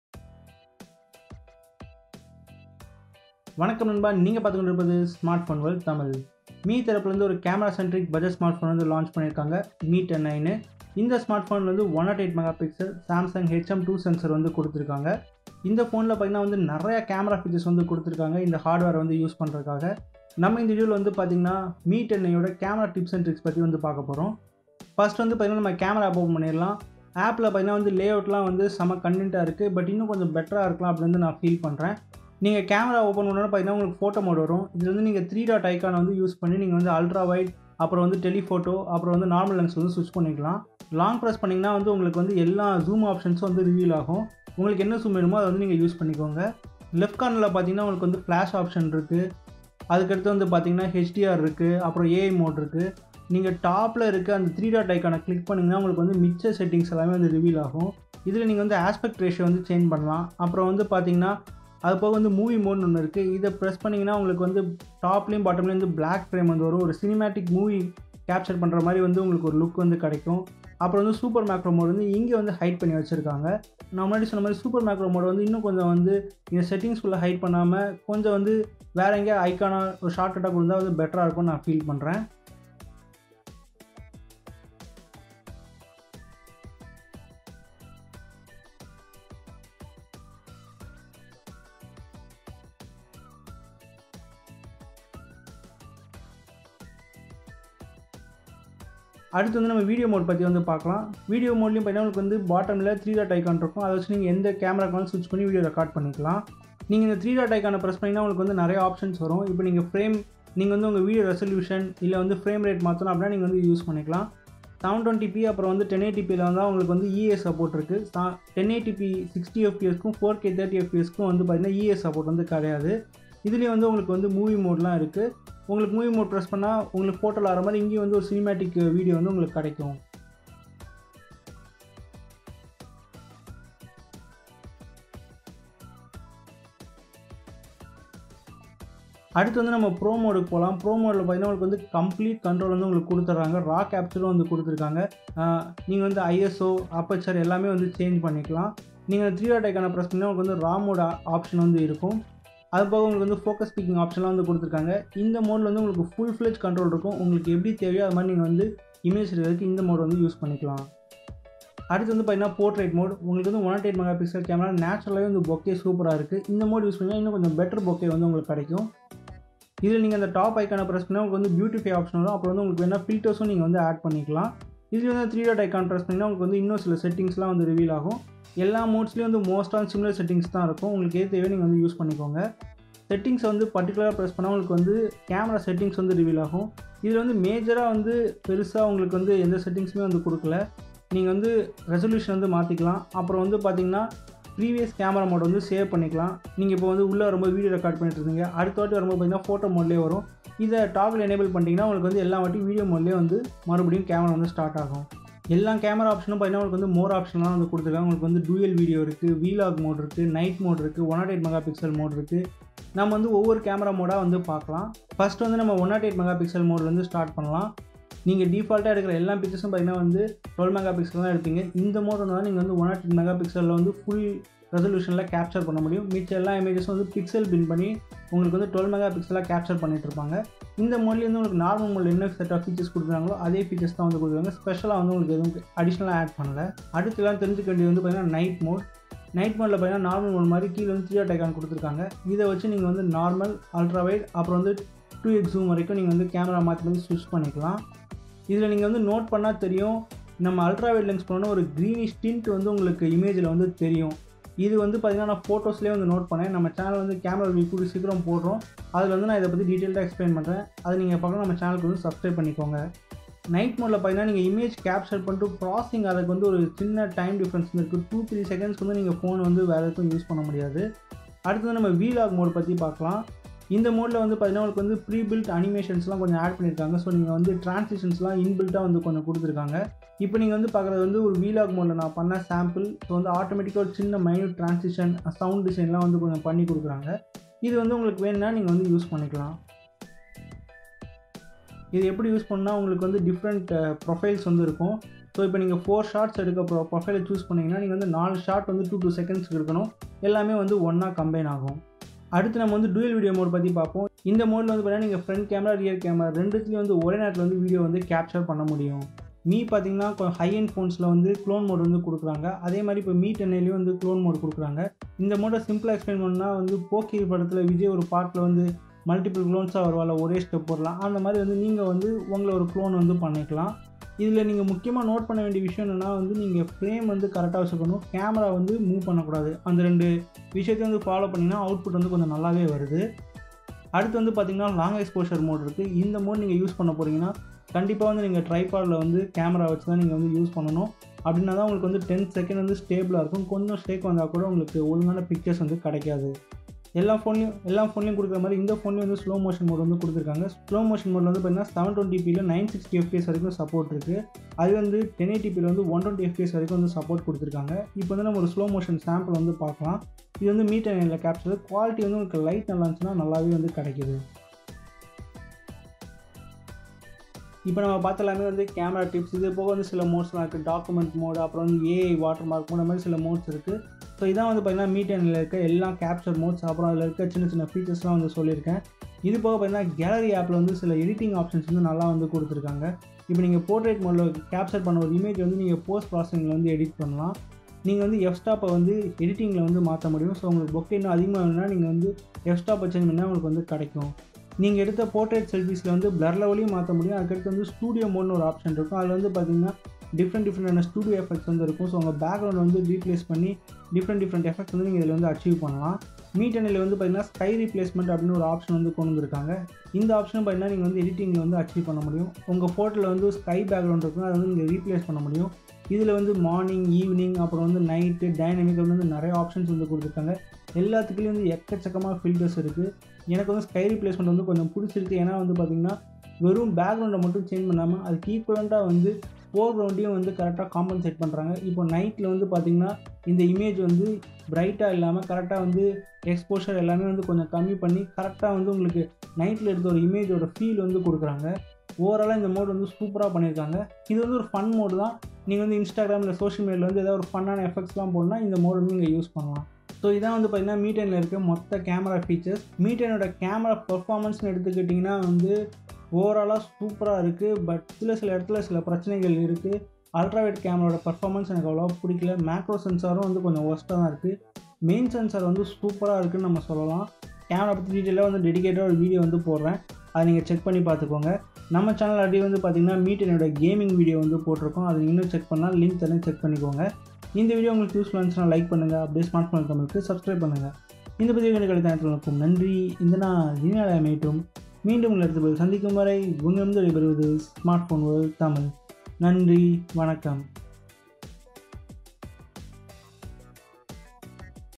Mi 10i camera tips and tricks tamil |Mi 10i 5G Camera settings features | Mi 10i camera test samples
Mi 10i 5G Camera tips and tricks tamil
    In this video we will take about Mi 10i camera settings and features. Mi 10i 5G camera features include AI sky replacement, 108MP mode, PRO mode, vlog mode, night mode and much more. I have also explained Mi 10i camera tips and tricks in tamil. Mi 10i 5G camera test samples and review in tamil is also done in this video. Mi 10i 5G tamil camera review video coming soon!!

0:00 - mi 10i camera intro
0:30 - mi 10i photo mode
1:48 - mi 10i video mode
2:25 - mi 10i pro mode
3:24 - mi 10i camera modes
4:00 - mi 10i pro mode